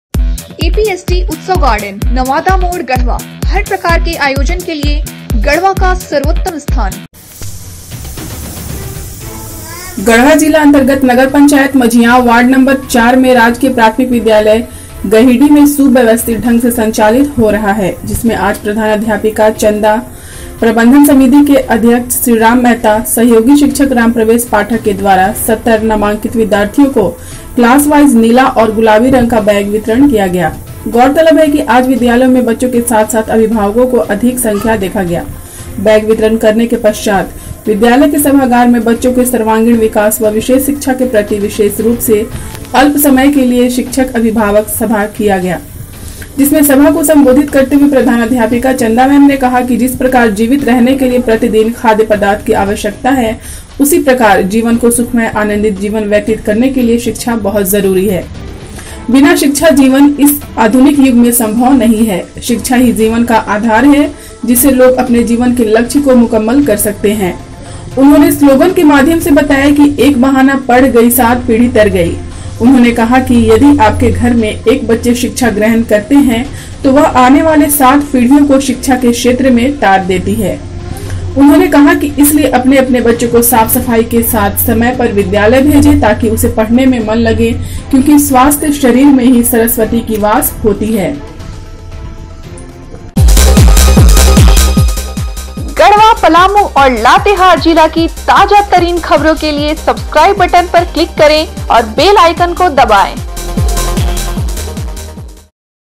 मझिआंव । गढ़वा । 70 विद्यार्थियों के बीच किया गया बैग का वितरण
मझिआंव । गढ़वा । गढ़वा जिला अंतर्गत नगर पंचायत मझिआंव वार्ड नं 04 में राजकीय प्राथमिक विद्यालय गहिडी में सुव्यवस्थित ढंग से संचालित हो रहा है। जिसमे आज प्रधानाध्यापिका चंदा मैं, प्रबन्धन समिति के अध्यक्ष श्रीराम मेहता, सहयोगी शिक्षक रामप्रवेश पाठक के द्वारा सत्तर नामांकित विद्यार्थियों को क्लाश वाइज नीला और गुलाबी रंग का बैग वितरण किया गया। गौरतलब है कि आज विद्यालयो में बच्चों के साथ साथ अभिभावकों को अधिक संख्या  देखा गया। बैग वितरण करने के पश्चात विद्यालय के सभागार में बच्चों के सर्वागीण विकाश व विशेष शिक्षा के प्रति विशेष रूप से अल्पसमय के लिए शिक्षक अभिभावक सभा किया गया। जिसमें सभा को सम्बोधित करते हुए प्रधानाध्यापिका चंदा मैम ने कहा कि जिस प्रकार जीवित रहने के लिए प्रतिदिन खाध पदार्थ की आवश्यकता है। उसी प्रकार जीवन को सुखमय आनंदित जीवन व्यतीत करने के लिए शिक्षा बहुत जरूरी है। बिना शिक्षा जीवन इस आधुनिक युग में सम्भव नहीं है। शिक्षा ही जीवन का आधार है। जिससे लोग अपने जीवन के लक्ष्य को मुकम्मल कर सकते हैं। उन्होंने स्लोगन के माध्यम से बताया कि एक बहाना पढ़ गईसात पीढ़ी तर गई।
उन्होंने कहा कि यदि आपके घर में एक बच्चे शिक्षा ग्रहण करते हैं तो वह आने वाले सात पीढ़ियों को शिक्षा के क्षेत्र में तार देती है।
उन्होंने कहा कि इसलिये अपने अपने बच्चों को साफ सफाई के साथ समय पर विद्यालय भेजे। ताकि उसे पढ़ने में मन लगे क्योकि स्वास्थ्य शरीर में ही सरस्वती की वास होती है।Talash TV
Latest News
Breaking News
Current News
garhwa news
palamu news
latehar news